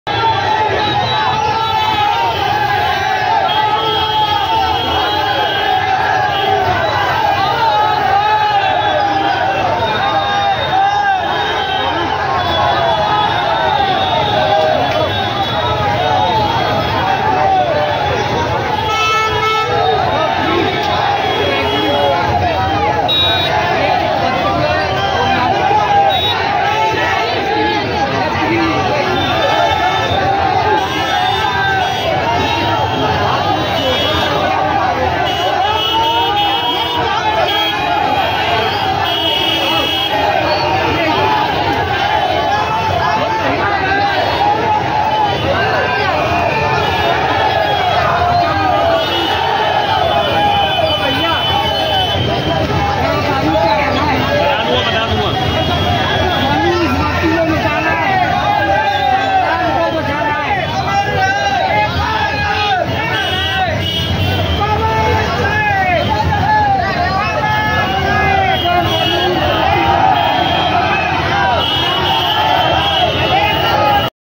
Bharat band 2024 Agra भारत बंद की सबसे बड़ी तस्वीर सिर्फ Ground Zero पर। रास्ता रोकने की क्या जरूरत
aaj ki taaja khabar. Bheem army ka Bharat band. tajmahal City Agra ka mahaul kya hai.